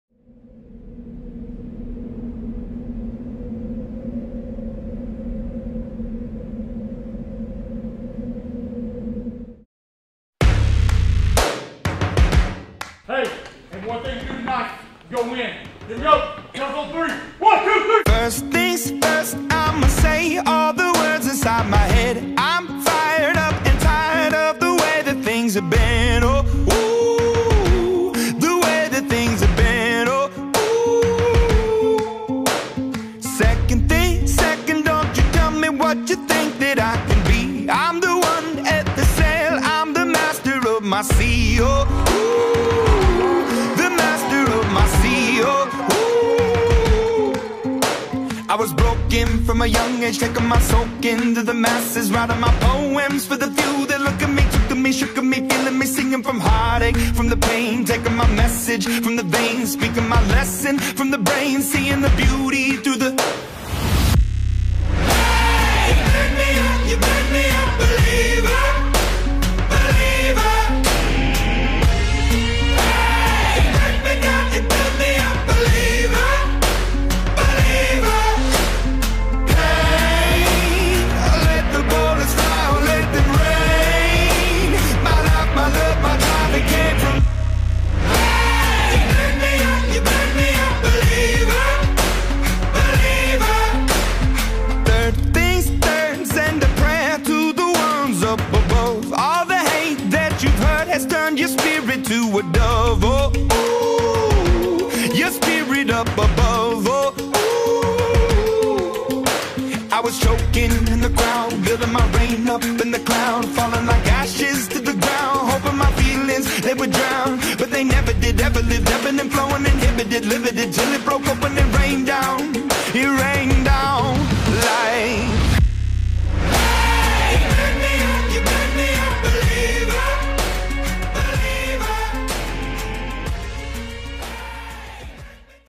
The Devils Den, Fear the Fork (Canon 80D, Football Hype Video)
The Williston Elko Blue Devils kick off their first home game of the 2018 Fall Season.

Special thanks to head football coach Derek Youngblood and all of the Blue Devil fans that came out to support the team.